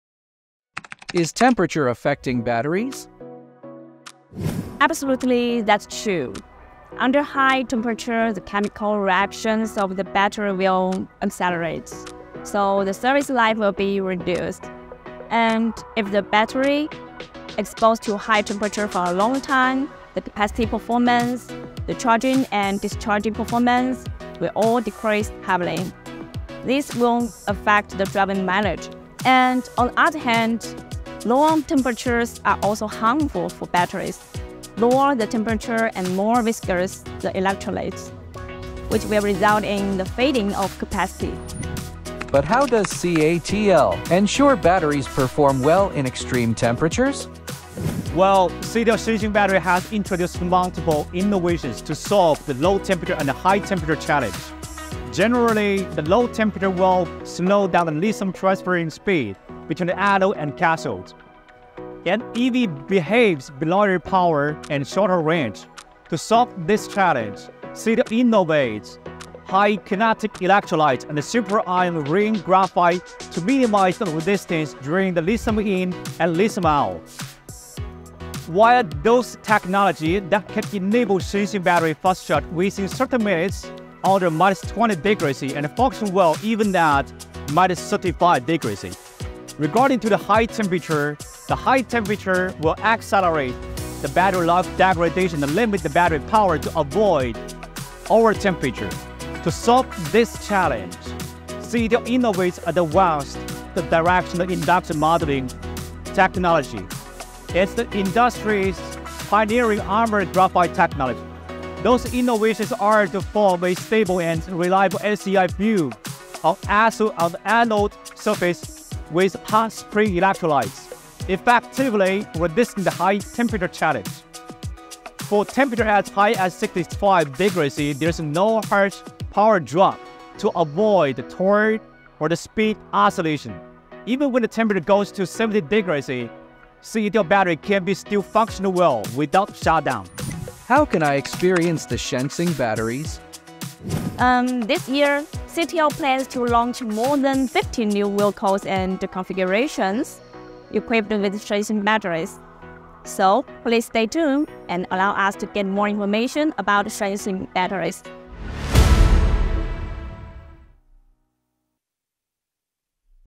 Engineer Q&A Blitz: Battery Safety
❓Is temperature affecting batteries? How does CATL ensure batteries perform well in extreme temperatures?
 👀Take a look at our video to find out the answers!